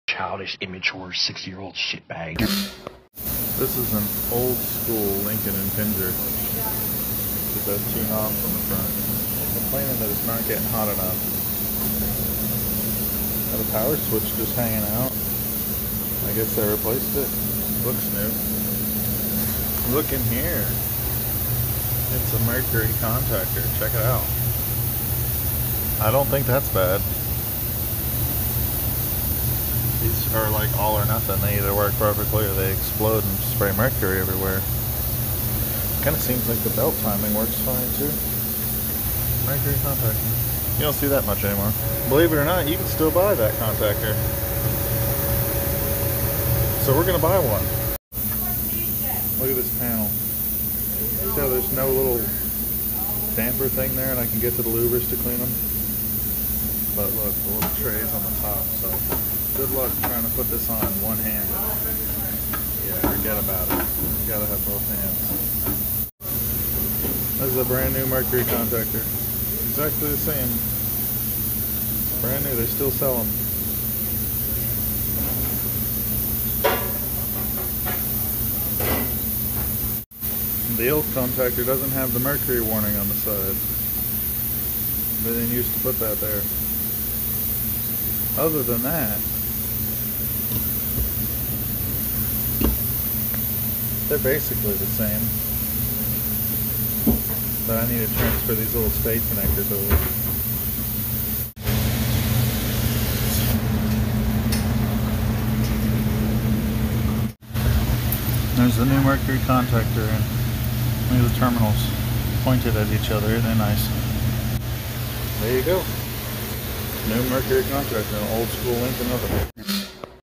Replacing A Mercury Contactor With A Mercury Contactor
A behind the scenes look at the art of installing a new mercury contactor